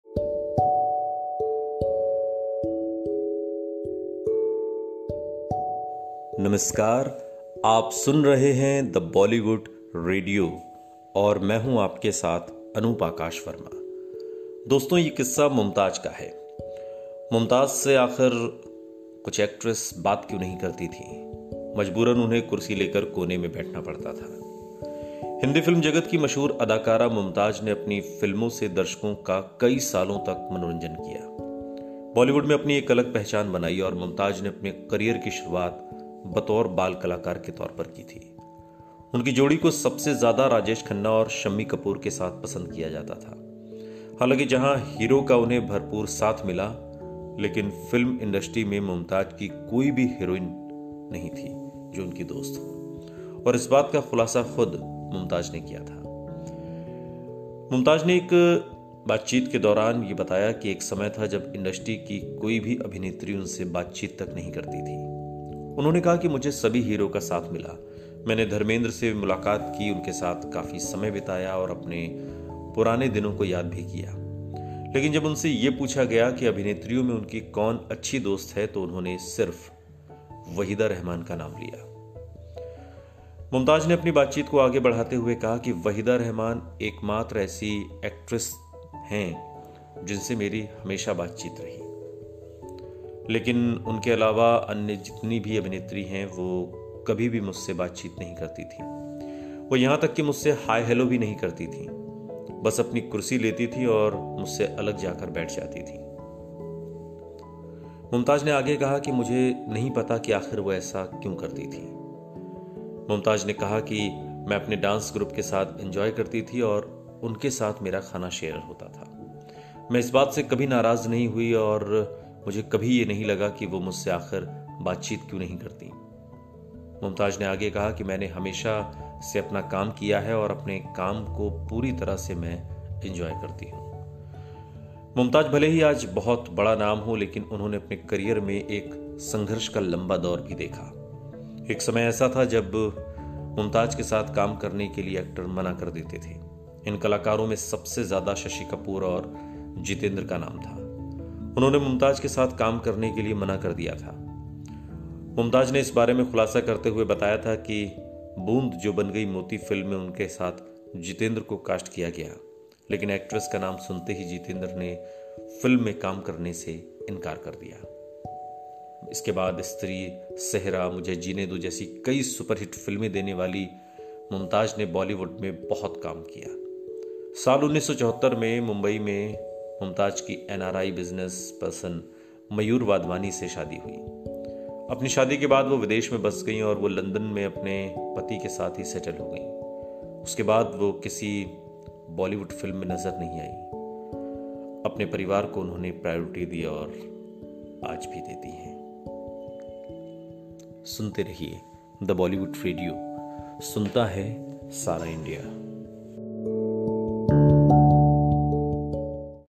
Mumtaz से बात क्यों नहीं करती थीं अभिनेत्रियां, कुर्सी लेकर कोने में अकेली बैठती थीं
हिन्दी फिल्म जगत की मशहूर अदाकारा मुमताज (Mumtaz) ने अपनी फिल्मों से दर्शकों का कई सालों तक मनोरंजन किया और बॉलीवुड में अपनी एक अलग जगह बनाई। मुमताज ने अपने करियर की शुरुआत बाल-कलाकार के रूप में की थी। उनकी जोड़ी को सबसे अधिक राजेश खन्ना और शम्मी कपूर के साथ पसंद किया जाता था। हालांकि जहां हीरो का उन्हें भरपूर साथ मिला, लेकिन फिल्म इंडस्ट्री में मुमताज की कोई भी हीरोइन नहीं थी। इस बात का खुलासा खुद मुमताज ने किया था. सुनिए पूरी कहानी का PODCAST अनूप आकाश वर्मा (Anoop Aakash Verma) के साथ. @thebollywoodradio